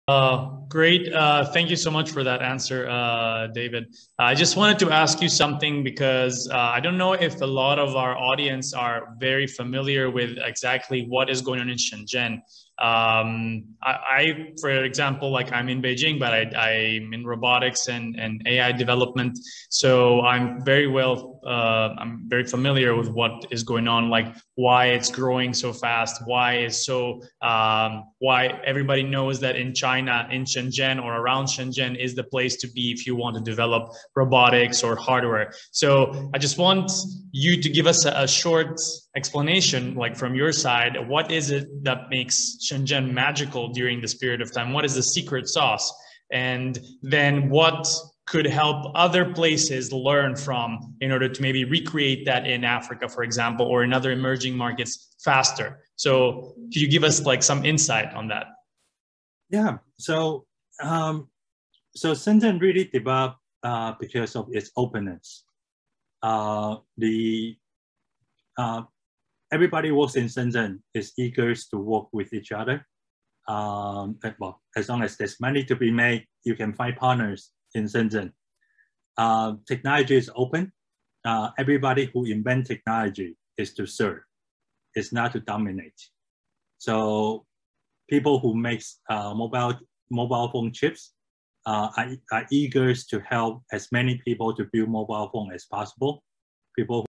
Why Shenzhen is a role model for other technology hubs
David Li, Robociti's August town hall guest speaker, explains why Shenzhen is a role model for other technological hubs and how emerging markets can attain this level.